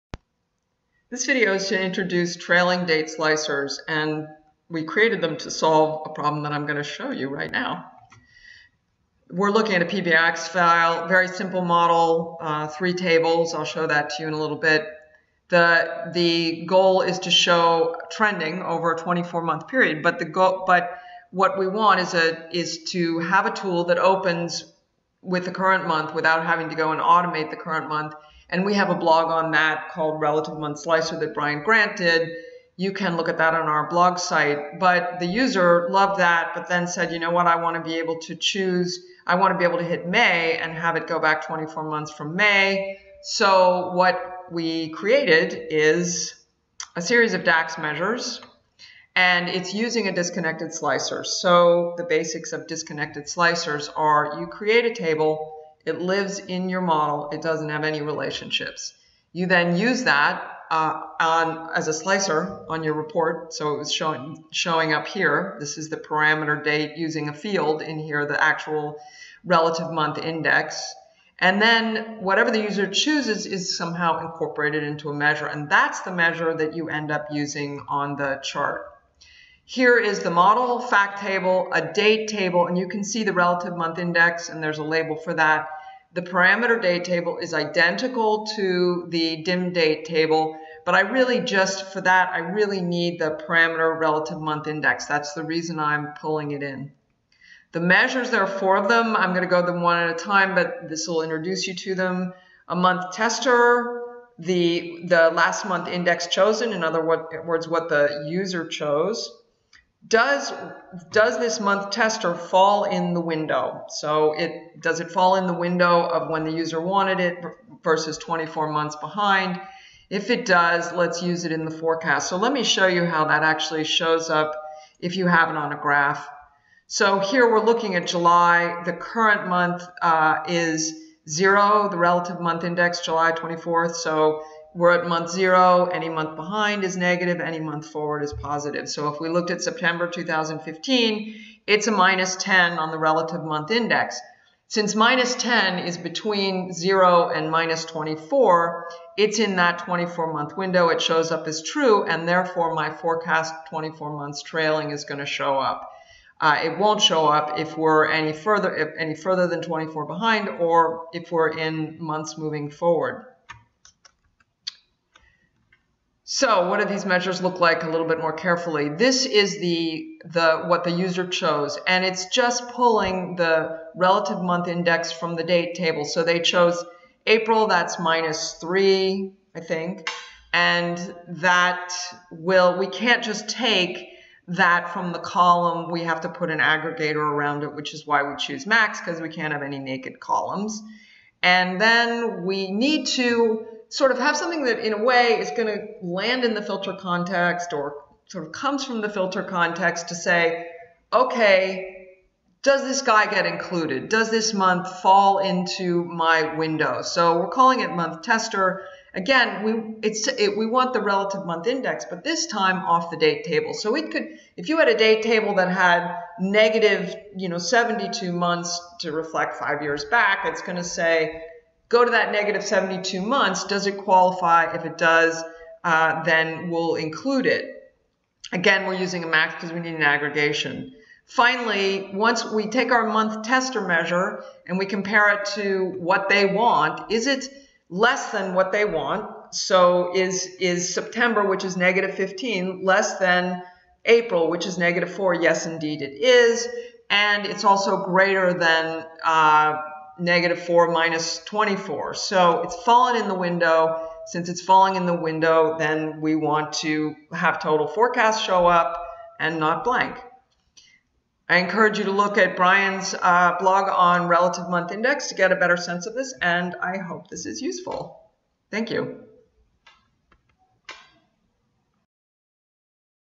Trailing Date Slicers in DAX for Power BI
Let users create slicers in Power BI models that look back over multiple months starting with months they get to choose. Uses DAX and a Disconnected Slicer.